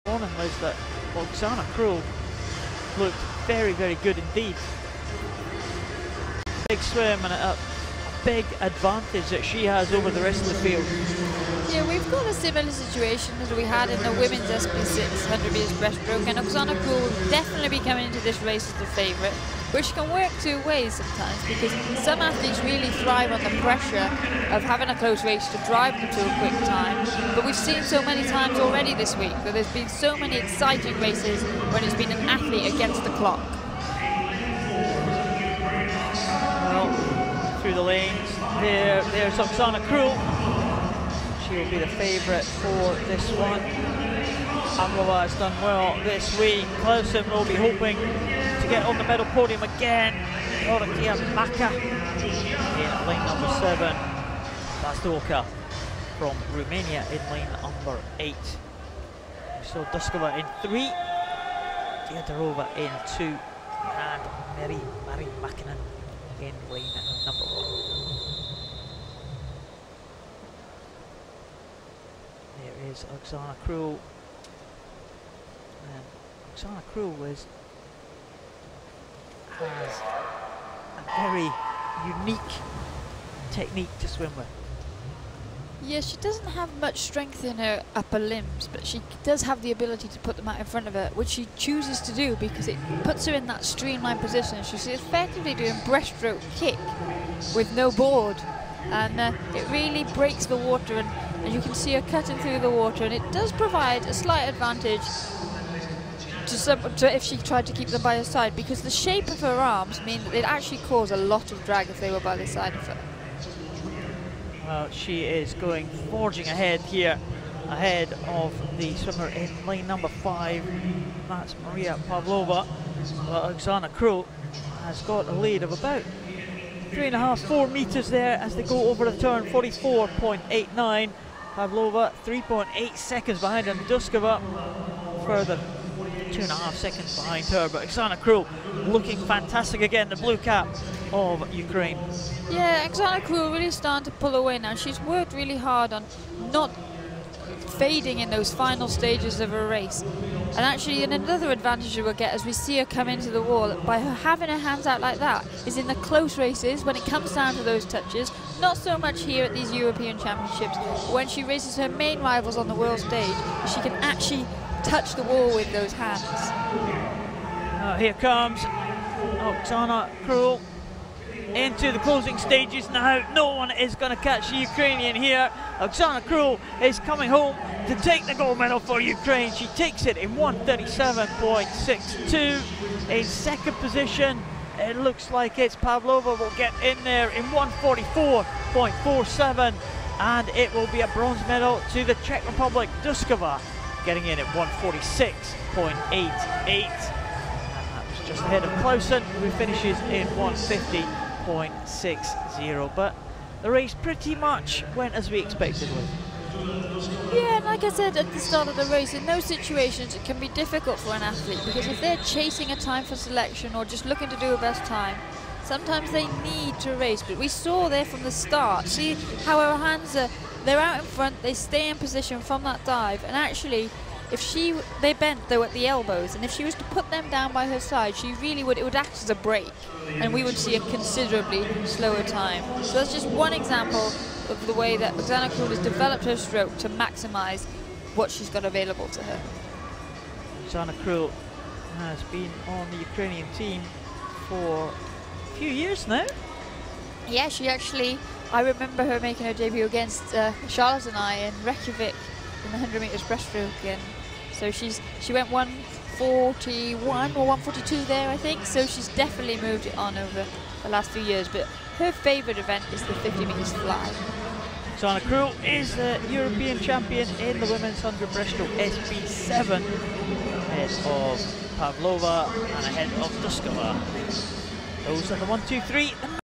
Women's 100m Breaststroke SB7 | Final | 2016 IPC Swimming European Open Championships Funchal
Gold: KHRUL Oksana Ukraine
Silver: PAVLOVA Mariia Russian Federation
Bronze: DUSKOVA Vendula Czech Republic

Start list:
MAKINEN Meri-Maari Finland
DIODOROVA Anastasia Russian Federation
DUSKOVA Vendula Czech Republic
KHRUL Oksana Ukraine
PAVLOVA Mariia Russian Federation
KLAUSEN Mina Marie Heyerdal Norway
BAKA Dorottya Hungary
BAZDOACA Georgiana Victoria Romania

453 athletes from 52 countries will compete in the 2016 IPC Swimming European Open Championships, the fourth edition of the competition.